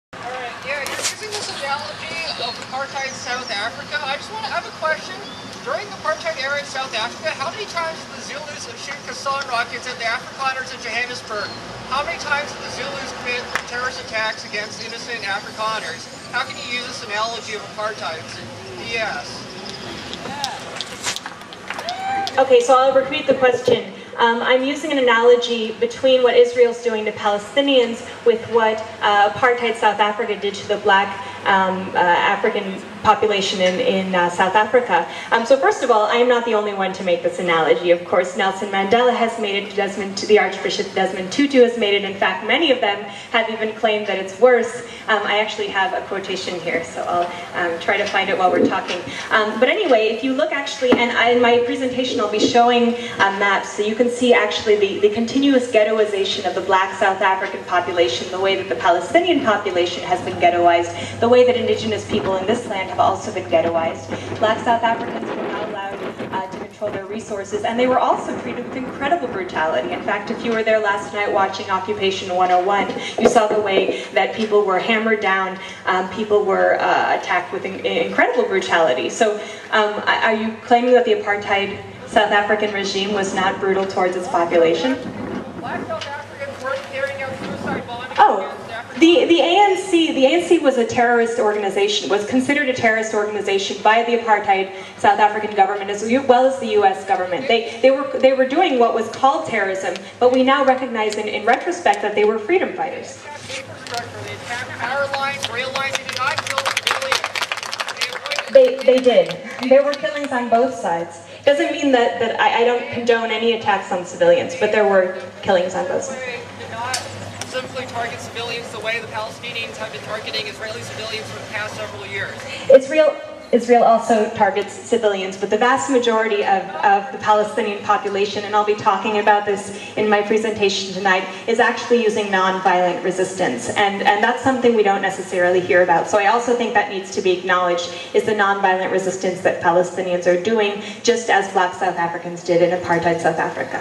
Witness in Palestine: Part I with Anna Baltzer Q&A 2
Witness in Palestine: Part I
with Anna Baltzer
Jewish American Columbia graduate and Fulbright Scholar
Granddaughter of Holocaust survivors
Author, "Witness in Palestine: A Jewish American Woman in the Occupied Territories"
Three-time volunteer with the International Women's Peace Service

May 12 // 12-1 PM // Flagpoles
At the University of California, Irvine

To those who critically analyze world news coverage, it is clear that the mainstream media often reports just one side of world issues. The Palestine-Israel conflict is no exception, as footage of attacks on innocent Israelis is shown repeatedly, yet the coverage of attacks on Palestinian civilians often misses the news. Anna Baltzer presents a first-hand look at the true condition of Palestine, and provides key insight into daily life under occupation from her own experiences in the territory.